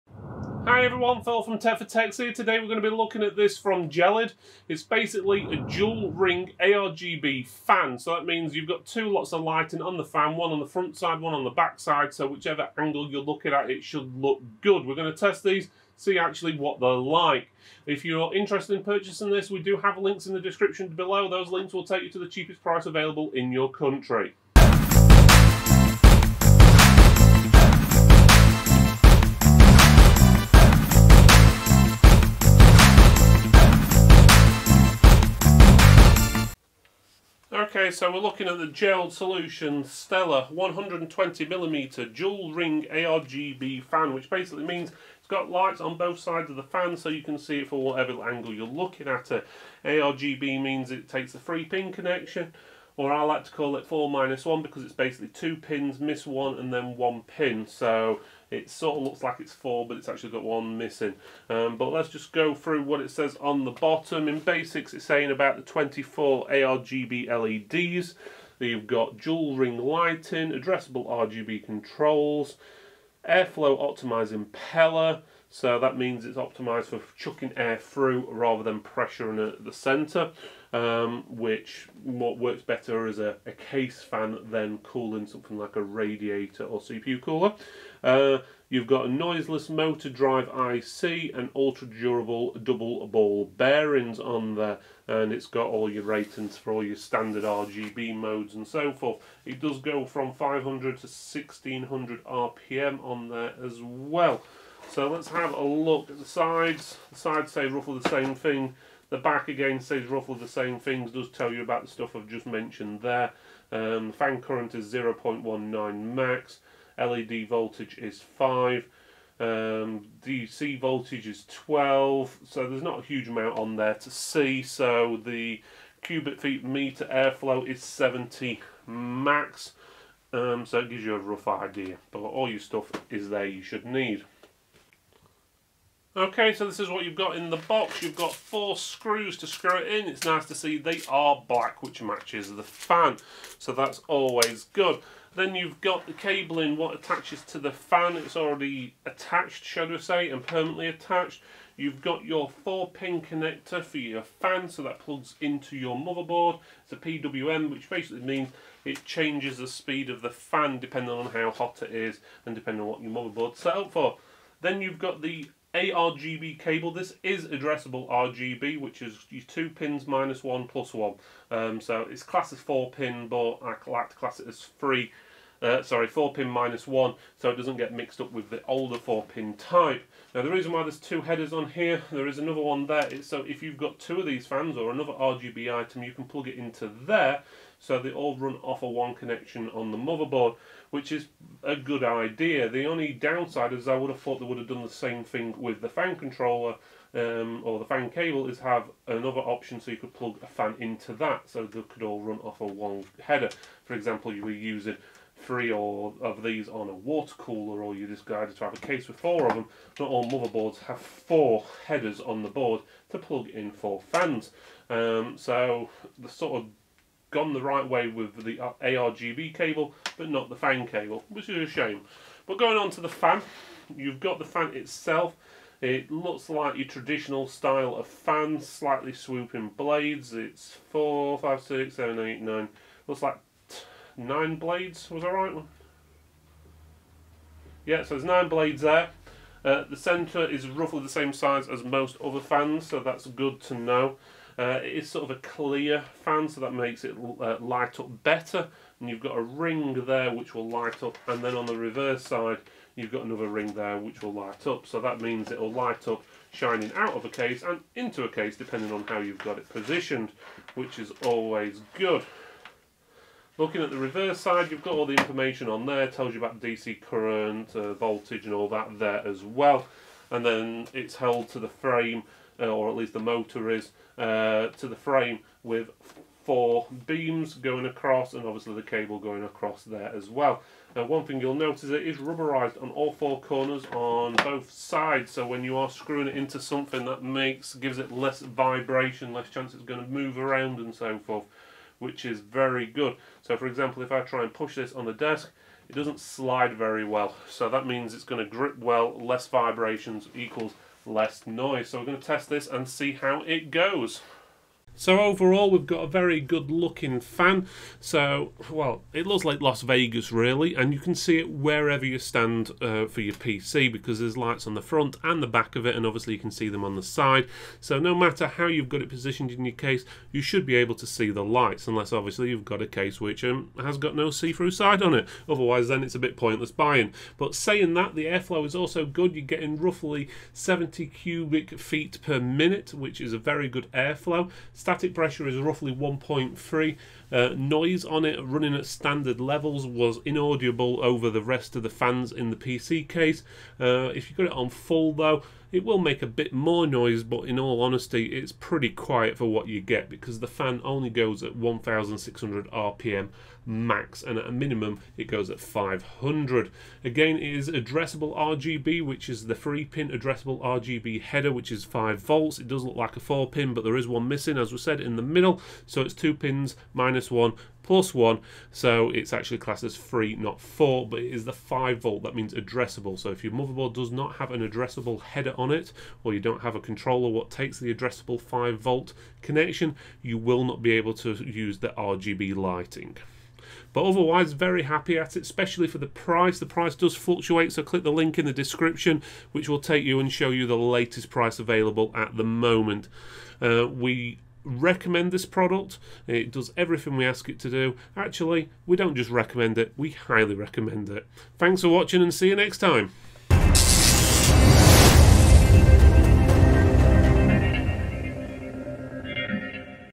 Gelid Stella, Dual Ring ARGB Airflow Fan - is this the best ARGB fan for under $10
Gelid Stella,  Dual Ring ARGB Airflow Fan (FN-STELLA-01)
Features:

● 24 ARGB LEDs
● Dual Ring Lighting
● Addressable RGB Controls
● Airflow Optimized Impeller
● Noiseless Motor Drive IC
● Ultra-Durable Double Ball Bearing

0:00 introduction
0:24 B-roll
0:36 Packaging
2:17 In the Box
5:05 Testing and conclusion

Category
Science & Technology